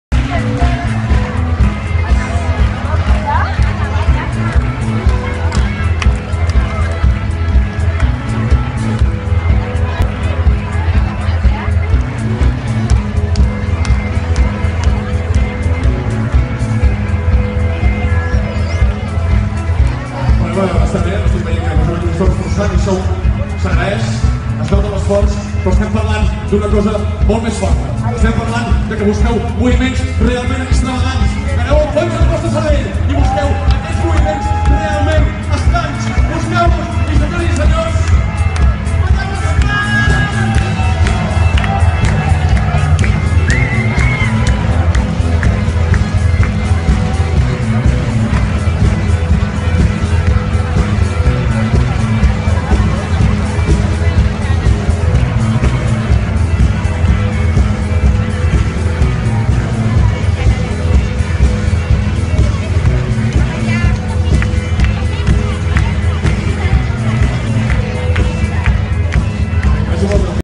Tastet de Manel a Petits Camaleons
Festival de música Petits Camaleons al Teatre-Auditori de Sant Cugat